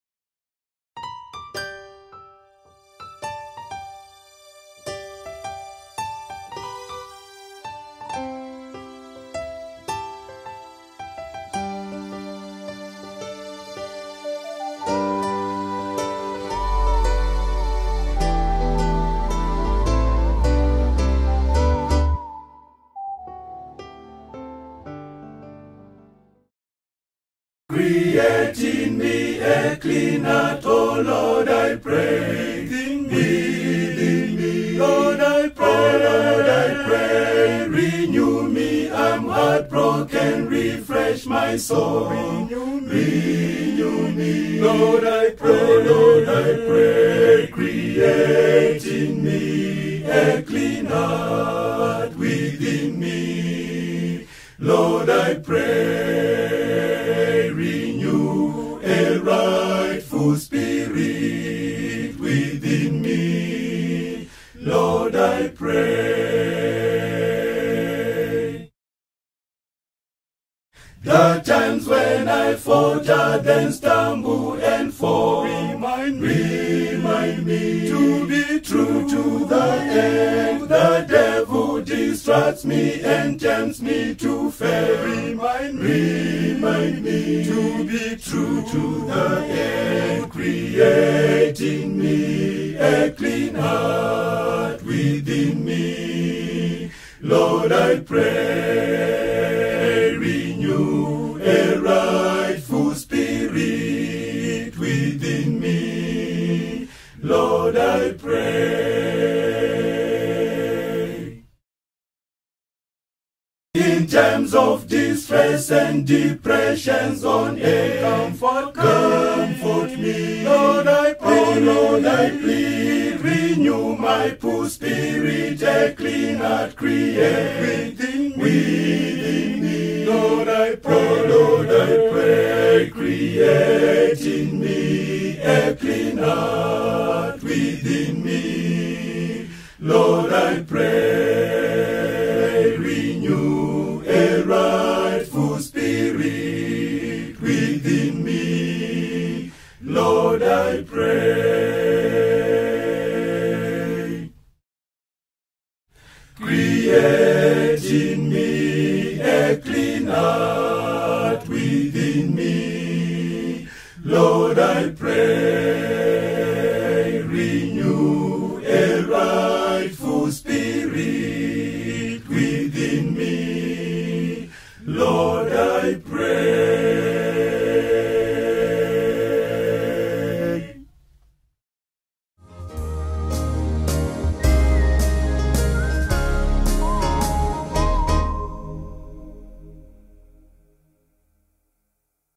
CREATE IN ME A CLEAN HEART
ONE'S FAVOURITE VERSE CAN BE PURE INSPIRATION. HERE IS M. MUHITA'S FAVOURITE VERSE.